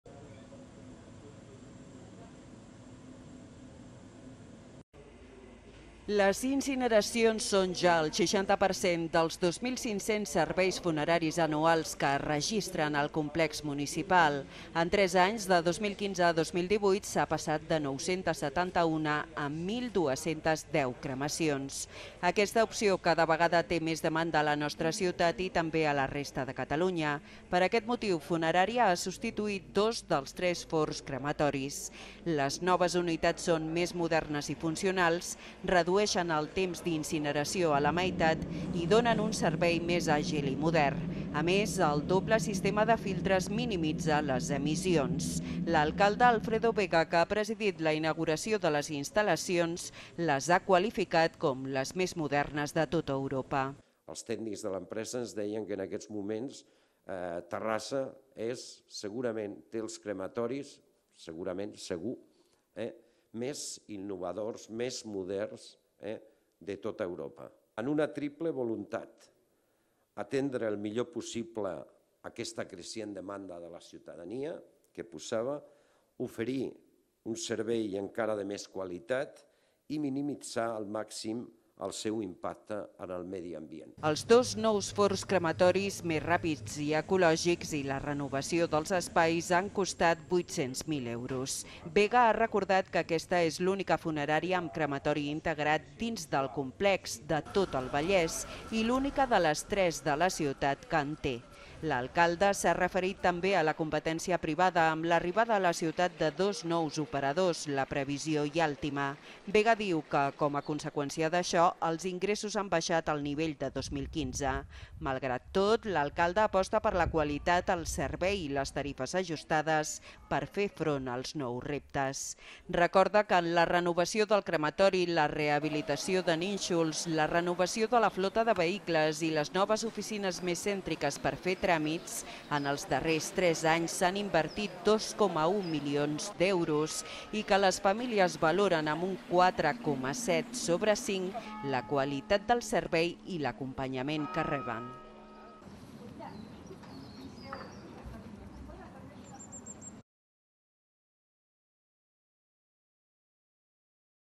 Els forns crematoris més moderns d’Europa, al Complex Funerari Municipal
Funerària ha substituït dos dels tres forns crematoris. Les noves unitats són més modernes i funcionals, redueixen el temps d’incineració a la meitat i donen un servei més àgil i modern. A més, el doble sistema de filtres minimitza les emissions.